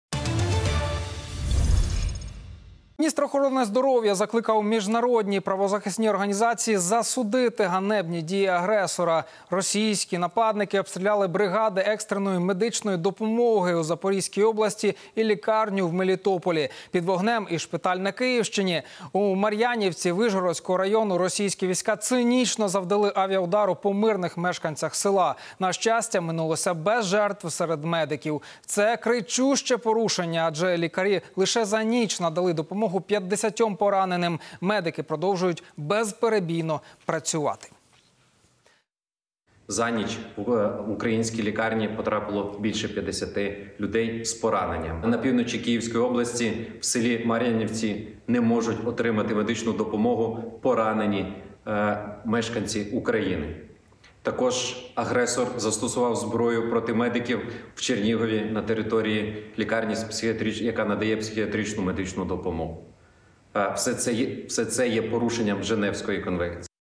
Загарбники обстріляли бригади екстреної медичної допомоги - Ляшко
Міністр охорони здоров’я закликав міжнародні правозахисні організації засудити ганебні дії агресора.

Російські агресори обстріляли бригади екстреної медичної допомоги у Запорізькій області та лікарню в Мелітополі.

Під вогнем і шпиталь на Київщині. У Мар'янівці Вишгородського району російські війська цинічно завдали авіаудару по мирних мешканцях села.

Побач більше!